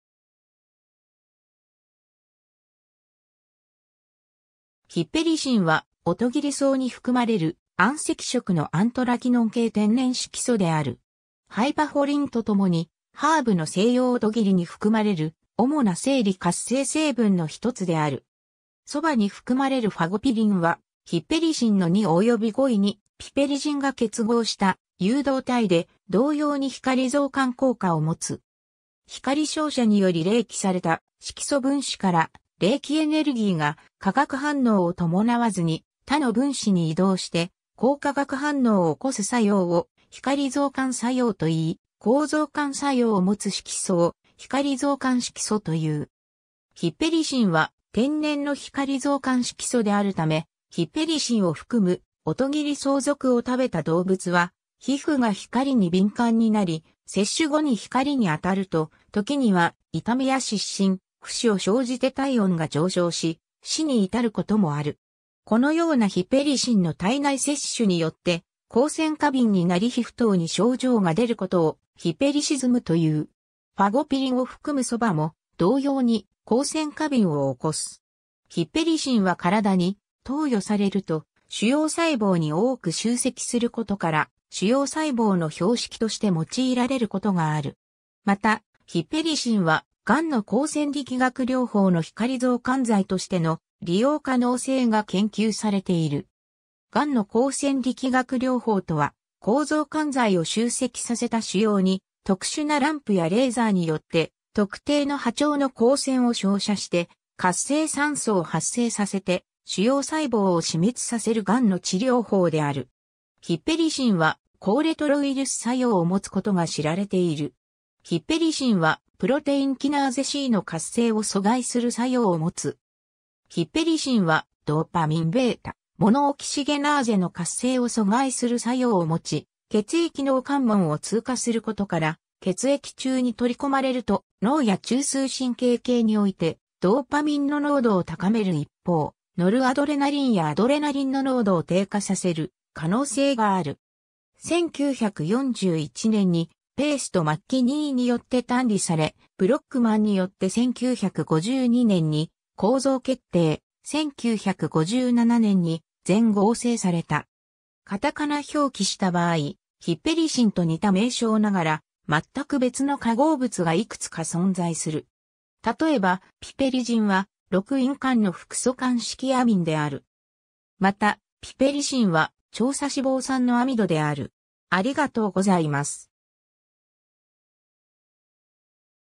ヒペリシン
ヒペリシン（hypericin）は、オトギリソウ (Hypericum) に含まれる暗赤色のアントラキノン系天然色素である。
ハイパフォリンとともにハーブのセイヨウオトギリ（セント・ジョーンズ・ワート）に含まれる主な生理活性成分の1つである。
 ソバに含まれるファゴピリン（Fagopyrine）はヒペリシンの2及び5位にピペリジンが結合した誘導体で、同様に光増感効果を持つ。
 光照射により励起された色素分子から励起エネルギーが化学反応を伴わずに他の分子に移動して光化学反応を起こす作用を光増感作用といい、光増感作用を持つ色素を光増感色素という。
 ヒペリシンは天然の光増感色素であるため、ヒペリシンを含むオトギリソウ属を食べた動物は皮膚が光に敏感になり、摂取後に光に当たると、ときには痛みや湿疹・浮腫を生じて体温が上昇し、死に至ることもある。
このようなヒペリシンの体内摂取によって光線過敏になり皮膚等に症状が出ることをヒペリシズム（hypericism）と言う。
ファゴピリンを含むソバも同様に光線過敏を起こす。
 ヒペリシンは体に投与されると腫瘍細胞に多く集積することから、腫瘍細胞の標識として用いられることがある。
また、ヒペリシンはがんの光線力学療法（PDT）の光増感剤としての利用可能性が研究されている。
がんの光線力学療法とは、光増感剤を集積させた腫瘍に特殊なランプやレーザーによって特定の波長の光線を照射して活性酸素を発生させて腫瘍細胞を死滅させるがんの治療法である。
 ヒペリシンは抗レトロウイルス作用をもつことが知られている。
 ヒペリシンはプロテインキナーゼC (PKC) の活性を阻害する作用をもつ。
 ヒペリシンはドーパミン-β-モノオキシゲナーゼ（DBH）の活性を阻害する作用を持ち、血液脳関門を通過することから、血液中に取り込まれると脳や中枢神経系においてドーパミンの濃度を高める一方、ノルアドレナリンやアドレナリンの濃度を低下させる可能性がある。
 1941年にペースとマッキニーによって単離され、ブロックマンによって1952年に構造決定、1957年に全合成された。
 片仮名表記した場合、ヒペリシン（Hypericin）と似た名称ながら、全く別の化合物が幾つか存在する。
例えば、ピペリジン（Piperidine）は、六員環の複素環式アミンである。
また、ピペリシン（Pipericine）は、長鎖脂肪酸のアミドである。